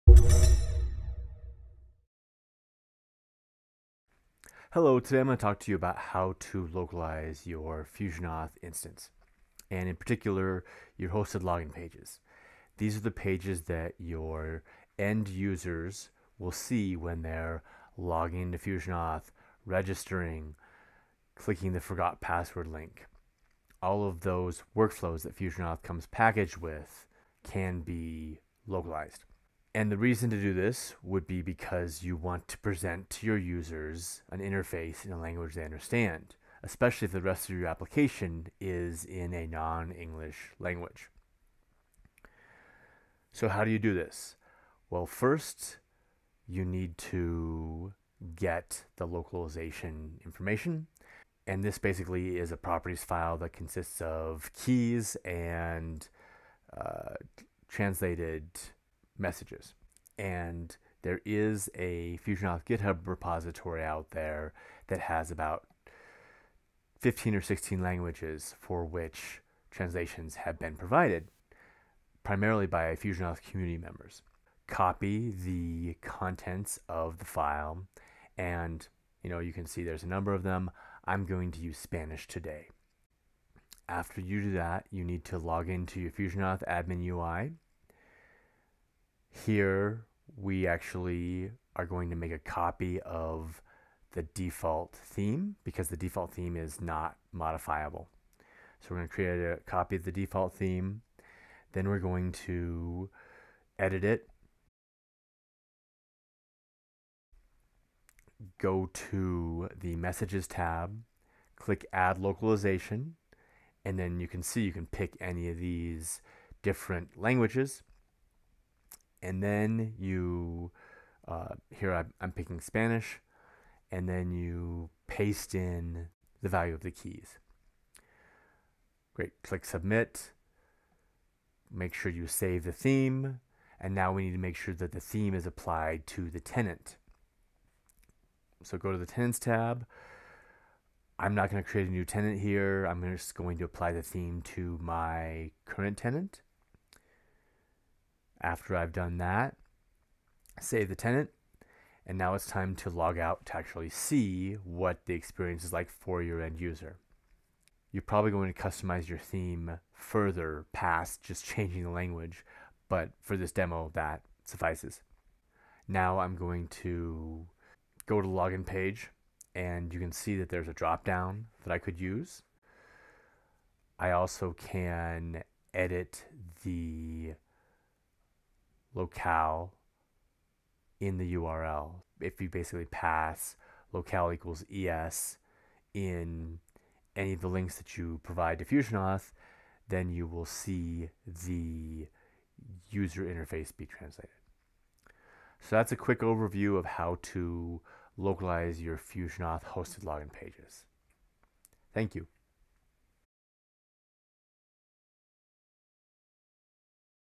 How to localize FusionAuth hosted login pages
FusionAuth provides out of the box login functionality, such as resetting passwords, login and registration. You can easily localize these pages to provide a better experience for your end users. This video shows how to use the Spanish translations for these pages.